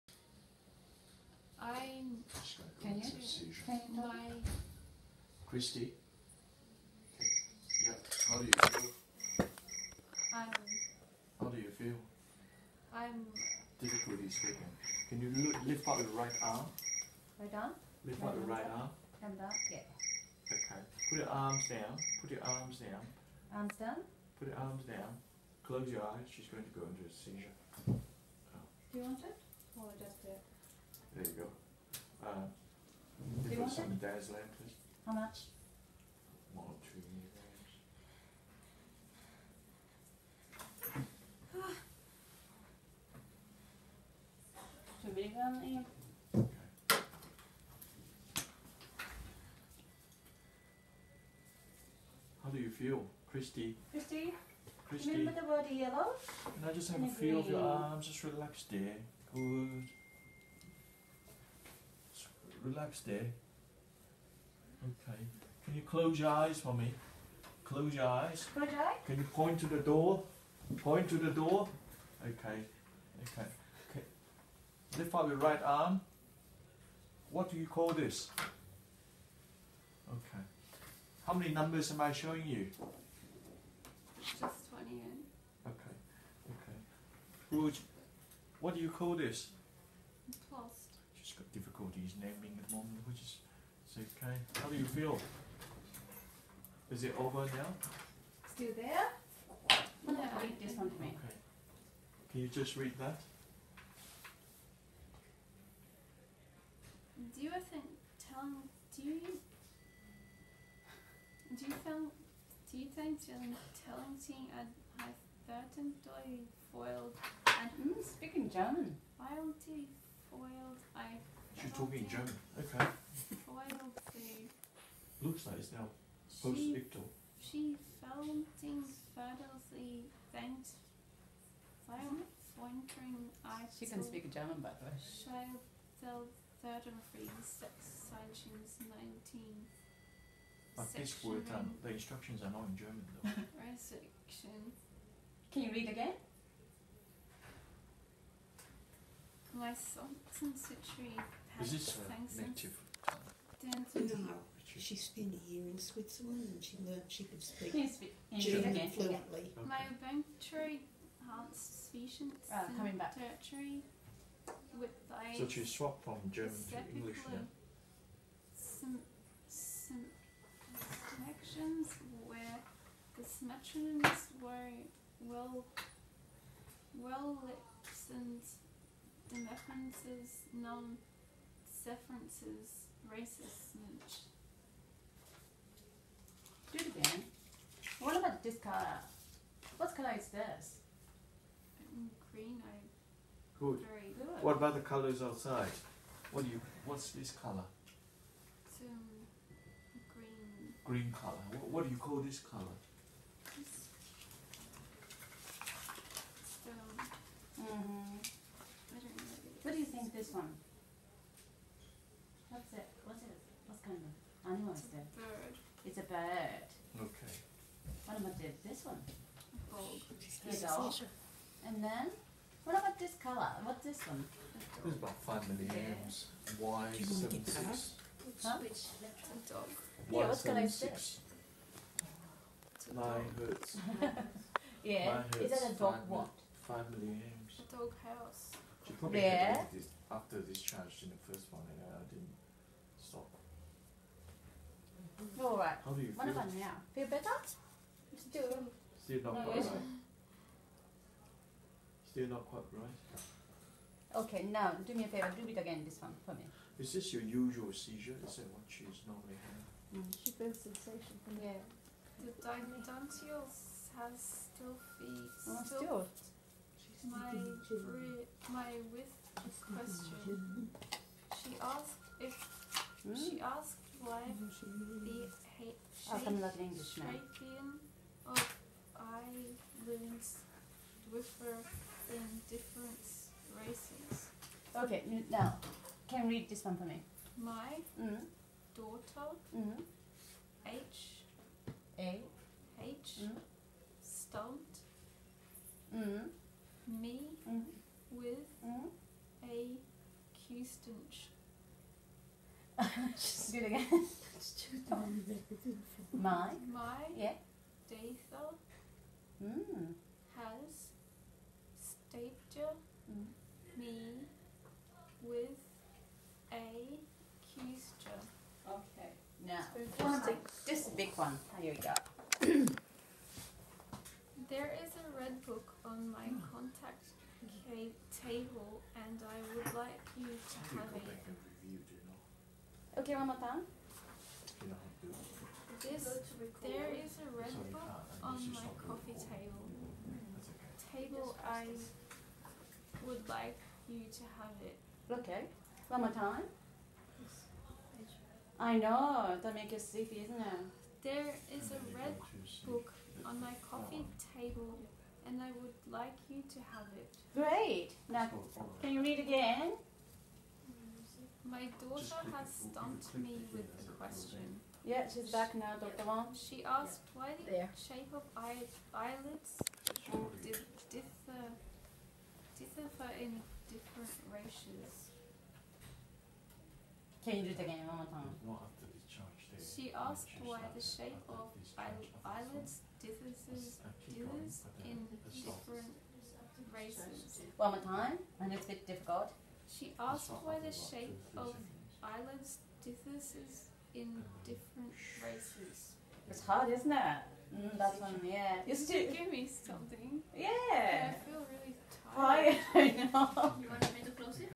Cortical Stimulation Stereotactic EEG Test for Epilepsy
Stereotactic EEG Test in Sydney, Australia. I had electrodes surgically implanted into my  brain (in the left temporal lobe) in order to locate my seizure focus (or foci).
In this test, they stimulate the electrodes in the different areas of my temporal lobe, and see if it elicits a seizure.
I think in this video, they had stimulated the electrode implanted in the posterior cingulate cortex of my left temporal lobe, and they recorded a focal dyscognitive seizure (I might be wrong about these details).
Before the nurse administered the midazolam to stop the seizure, I experienced an overwhelming and intolerable feeling of horror. It’s hard to find the words to describe how it felt. You can see when I start to cringe and flinch. After that point I don’t remember anything. So, I have no recollection of me reading and trying to name things.
And no, I wasn’t speaking German! That’s just gibberish! o_O
(I did learn German in Switzerland, though ;)